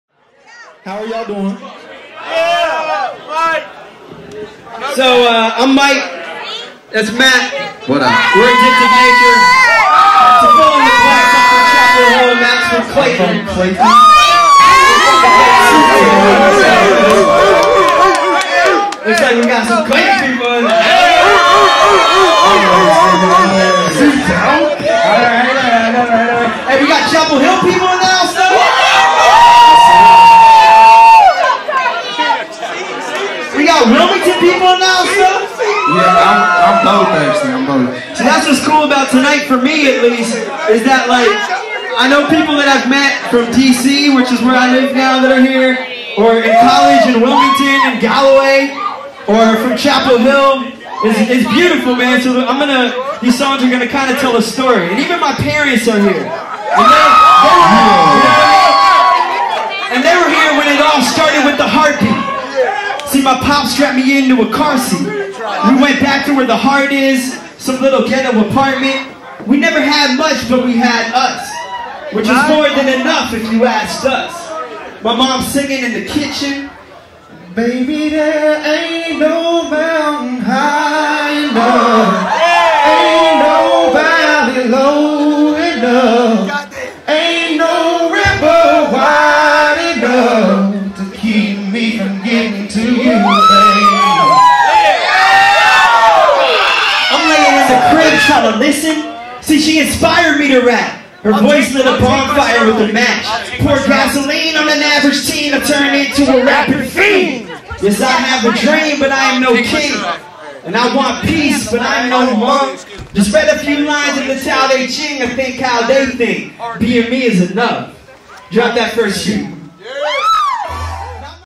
Addictive Nature - Live Intro
Intro to our set in Raleigh, NC at Getdownson's Break Thru @ the  Ballroom event. 2014. Inahle life, exhale music.